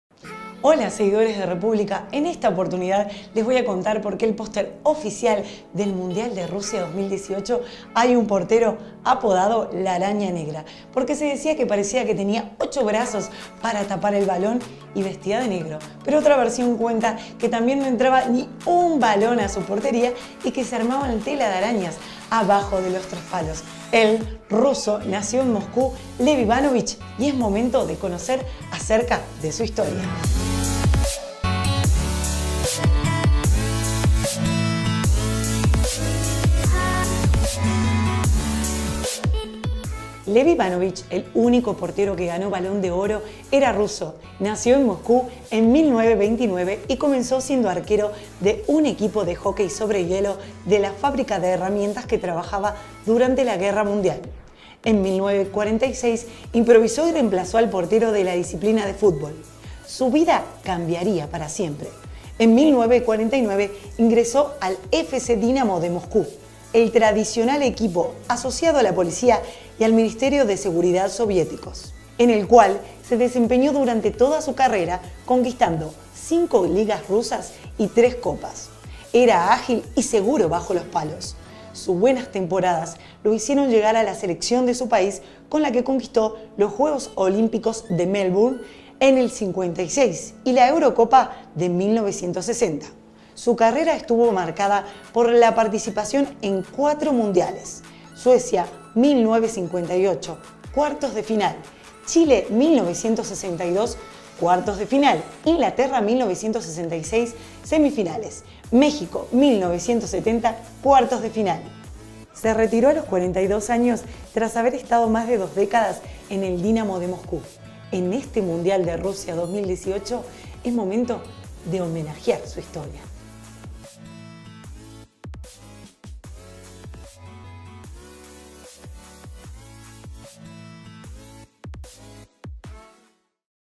LA HISTORIA DE LEV IVÁNOVICH Y EL POR QUÉ DEL POSTER DEL MUNDIAL RUSIA 2018
Te contamos por qué el póster oficial del Mundial de Rusia 2018 homenajea al legendario portero soviético Lev Yashin, apodado “La Araña Negra”.